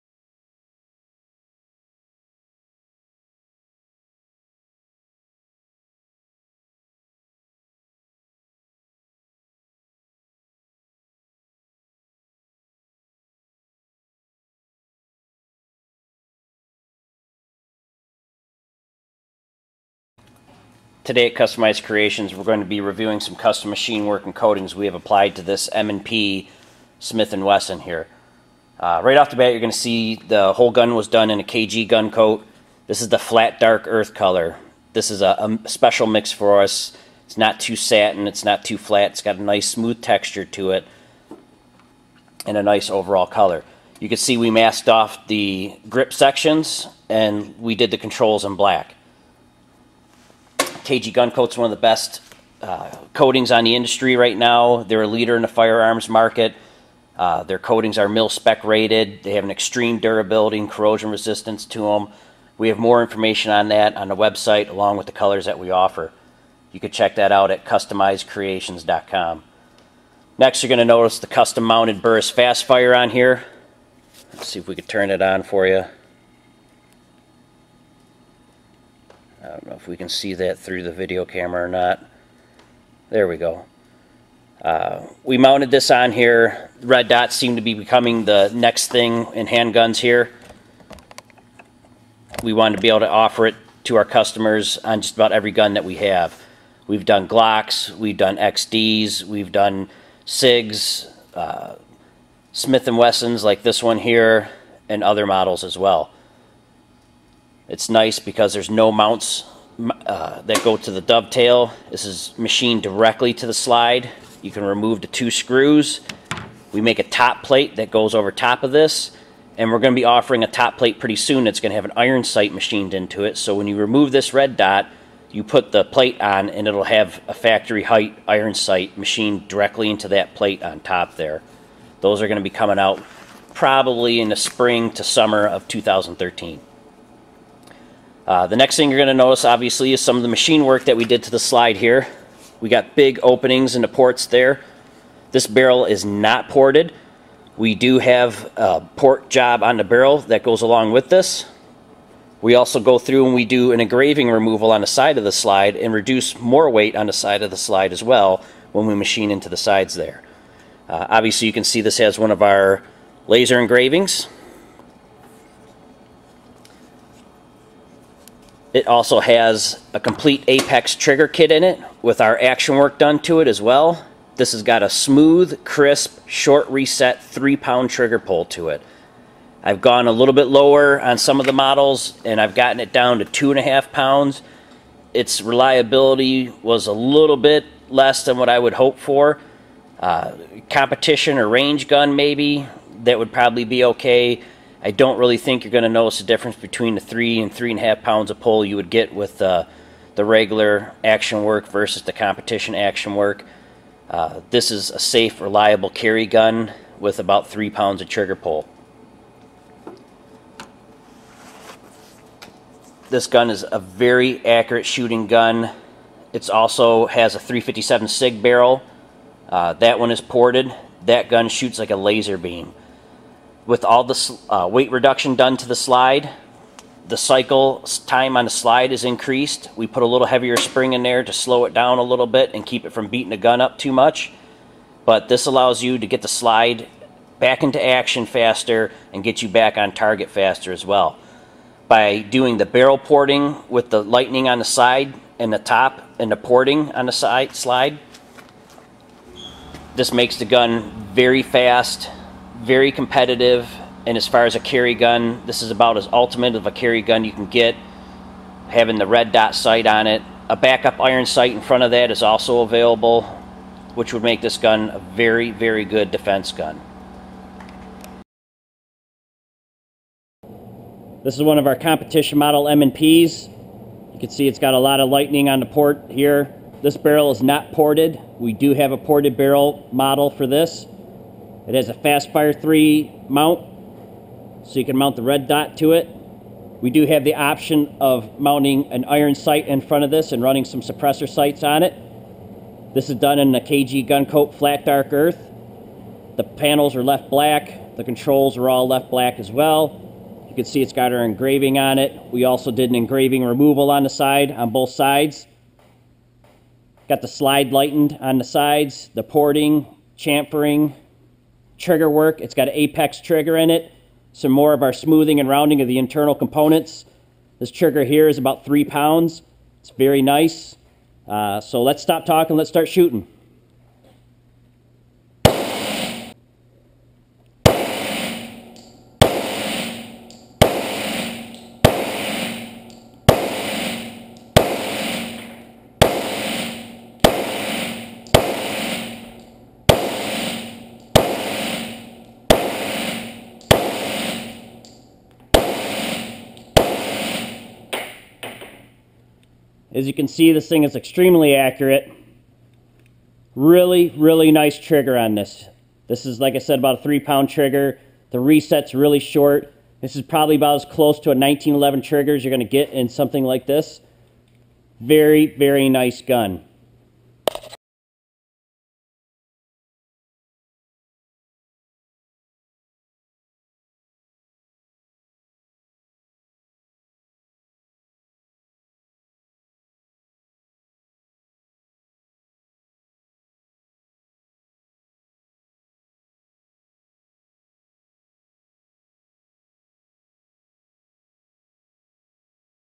Customized Creationz Custom Smith Wesson M&P Machine Work and Refinishing
Customized Creationz Custom Smith and Wesson M&P Machine Work and Refinishing. Featured is a competition model M&P with custom slide lightening and porting on the sides and top of the slide. Also a complete 3.0 lb trigger pull using Apex trigger and our action enhancement package. Machined for a Burris Fast Fire 3 Red Dot system. Finally refinished in KG Gunkote's Flat Dark Earth and Stealth Black coatings. Last but not least the finishing touch is our Company logo laser engraved into the side of the slide.